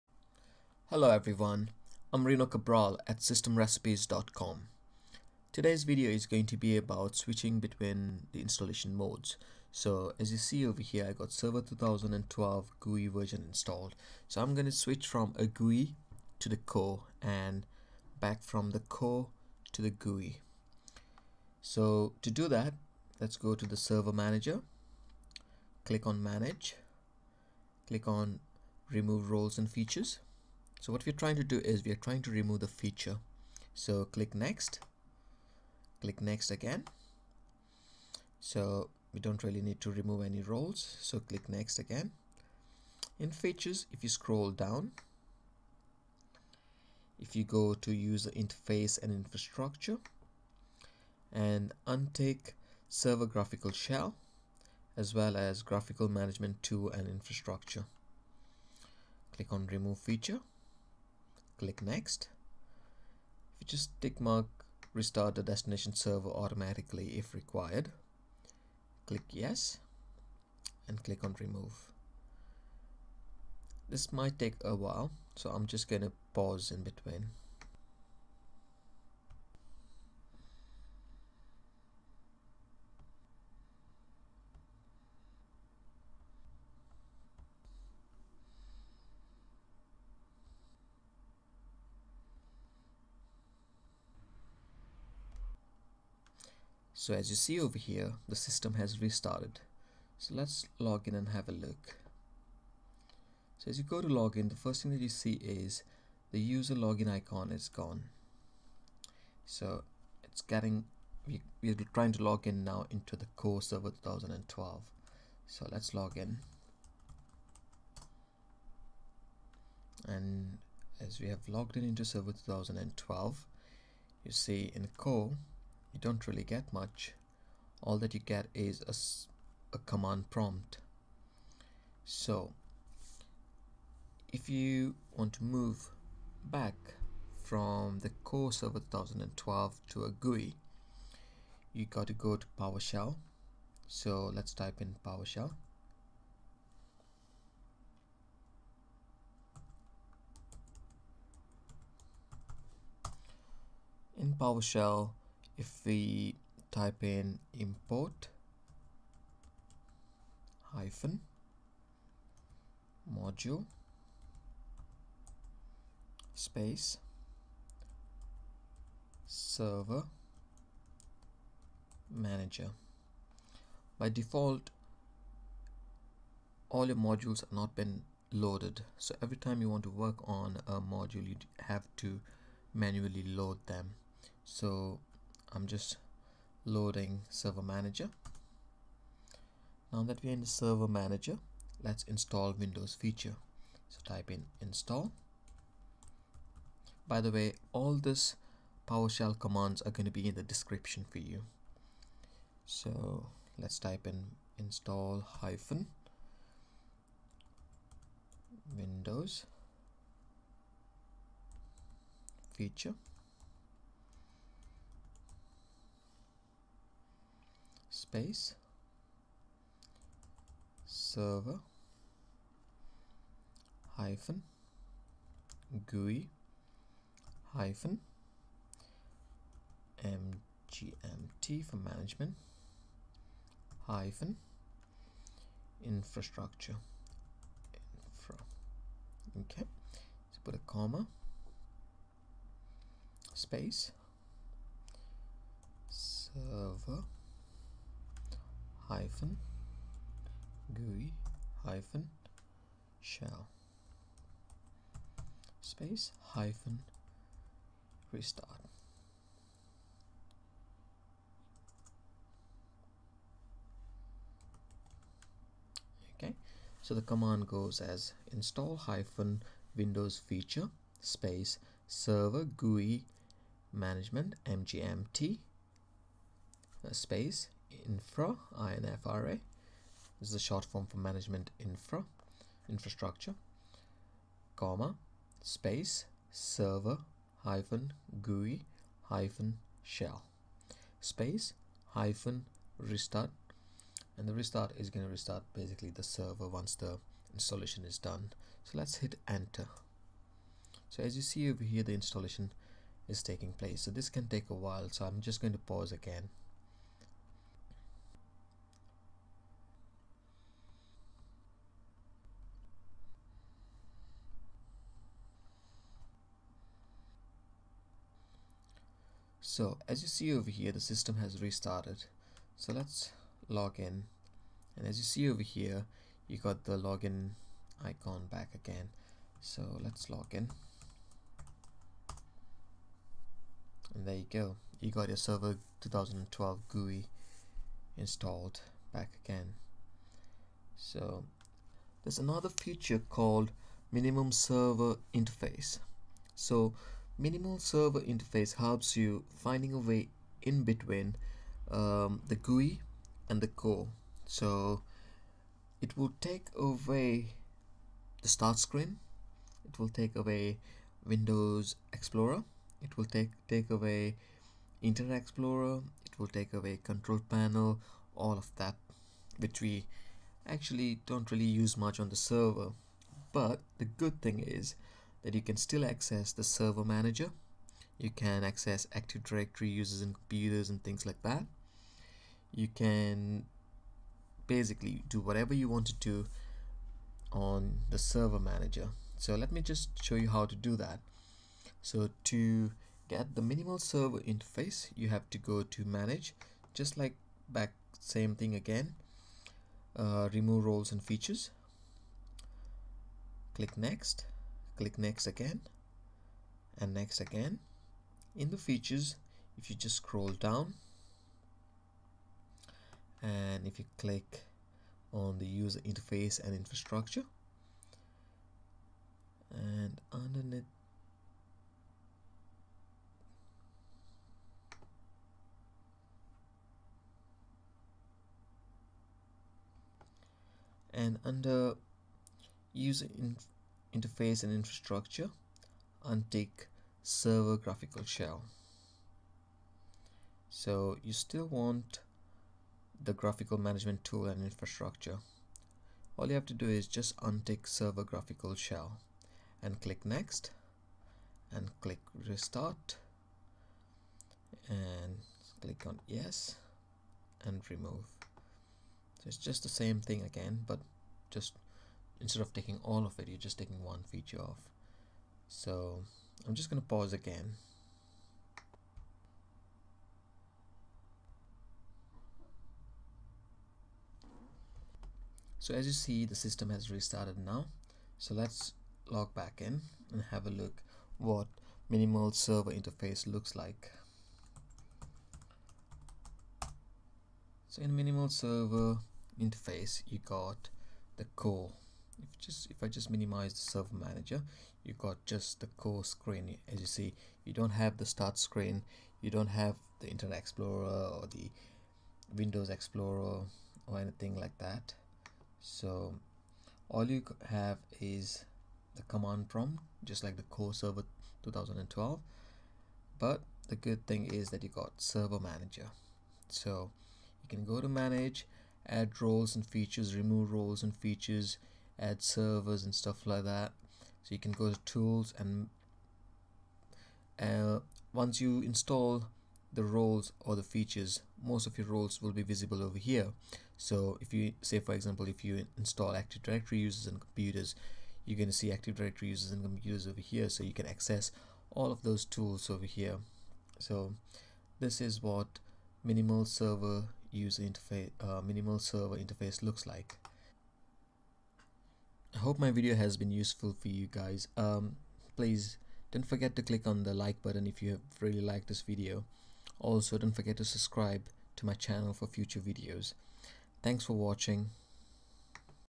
Windows Server 2012 - Switching installation modes between GUI and Server Core
This video would demonstrate a new Windows Server 2012 feature to dynamically switch between the default installation mode, Server Core, and the more traditional mode, GUI (Graphical User Interface) mode.